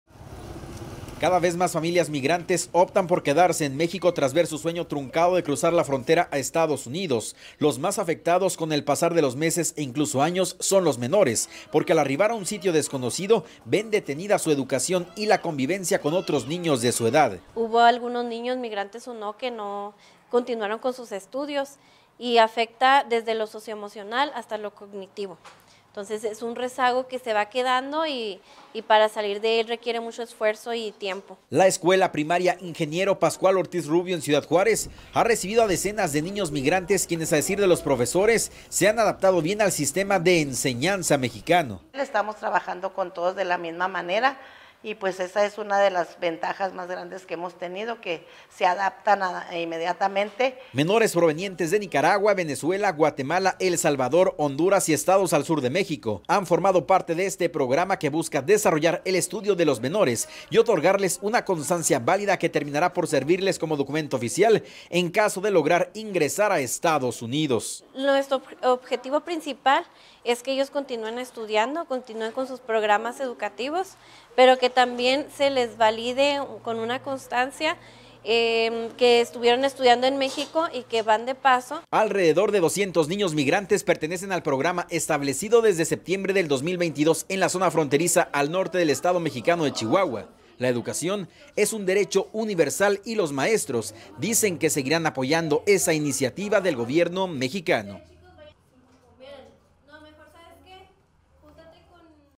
Escuela apoya a niños migrantes en Ciudad Juárez
Familias migrantes que se encuentran varadas en Ciudad Juárez, México, en la frontera con EEUU, encuentran apoyo para la educación de sus hijos.